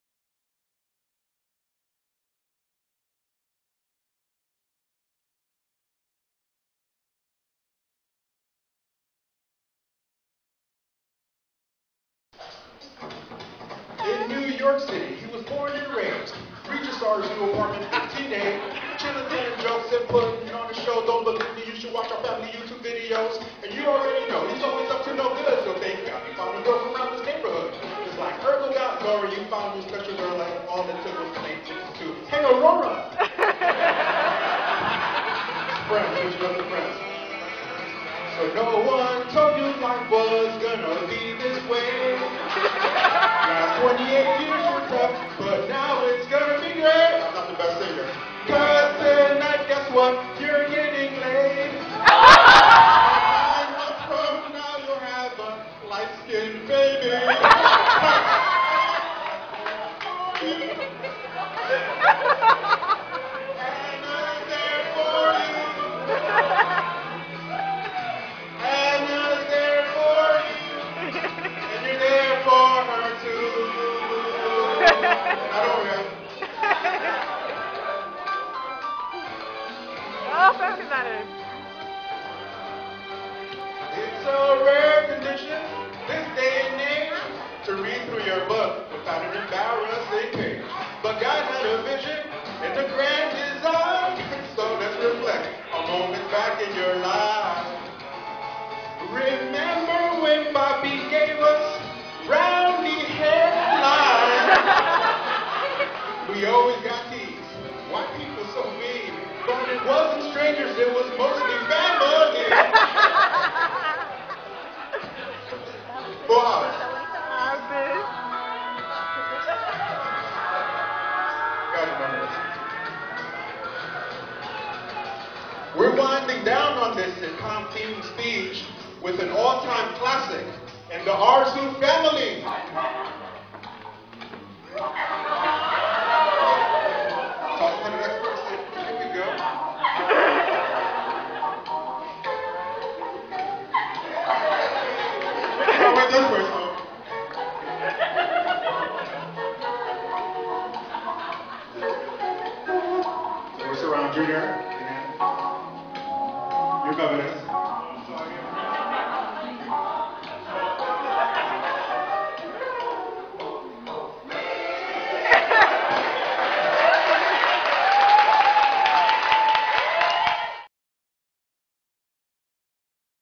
Arzu Best Man Toast
90's sitcom themed best man toast.